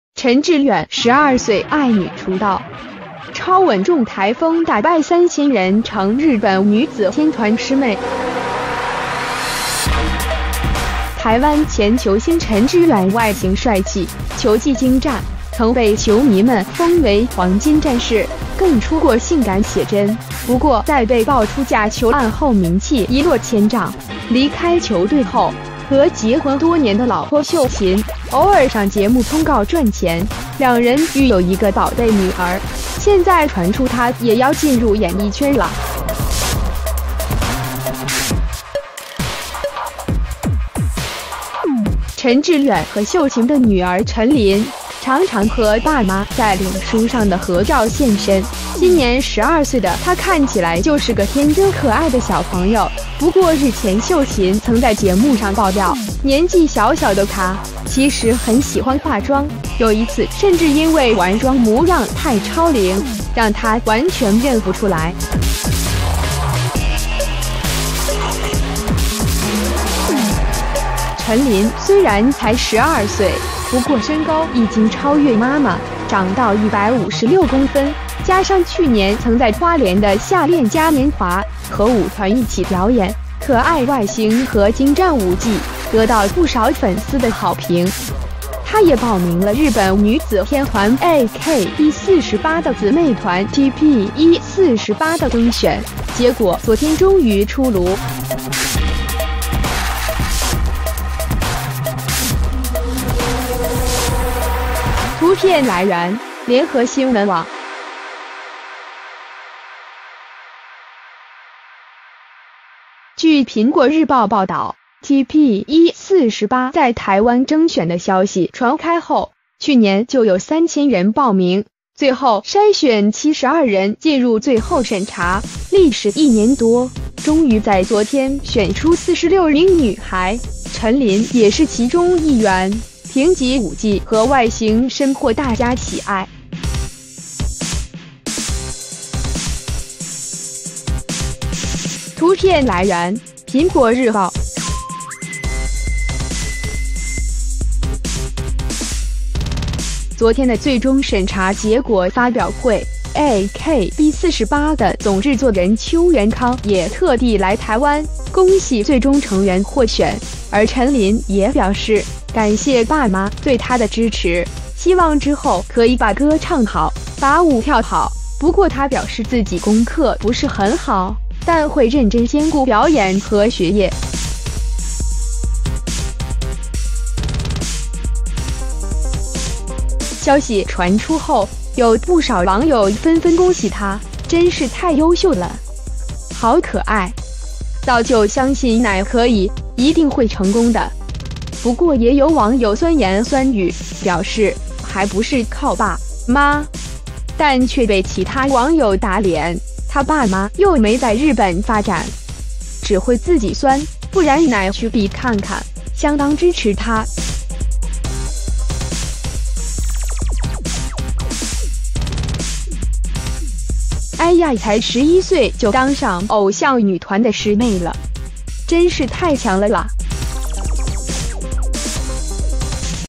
陳致遠「12歲愛女」出道！超穩重台風「打敗3000人」成日本女子天團師妹！
台灣前球星陳致遠外型帥氣、球技精湛，曾被球迷們封為「黃金戰士」，更出過性感寫真，不過在被爆出假球案後名氣一落千丈，離開球隊後，和結婚多年的老婆秀琴，偶爾上節目通告賺錢，兩人育有一個寶貝女兒，現在傳出她也要進入演藝圈啦！

陳致遠和秀琴的女兒陳琳，常常和爸媽在臉書上的合照現身，今年12歲的她看起來就是個天真可愛的小朋友，不過日前秀琴曾在節目上爆料，年紀小小的她，其實很喜歡化妝，有一次甚至因為完妝模樣太超齡，讓她完全認不出來！

陳琳雖然才12歲，不過身高已經超越媽媽，長到156公分，加上去年曾在花蓮的夏戀嘉年華，和舞團一起表演，可愛外型和精湛舞技，得到不少粉絲的好評！她也報名了日本女子天團AKB48的姊妹團TPE48的徵選，結果昨天終於出爐！

圖片來源：《聯合新聞網》

據《蘋果日報》報導，TPE48在台灣徵選的消息傳開後，去年就有3000人報名，最後篩選72人進入最後審查，歷時1年多，終於在昨天選出46名女孩，陳琳也是其中一員，憑藉舞技和外型深獲大家喜愛！

圖片來源：《蘋果日報》

昨天的最終審查結果發表會，AKB48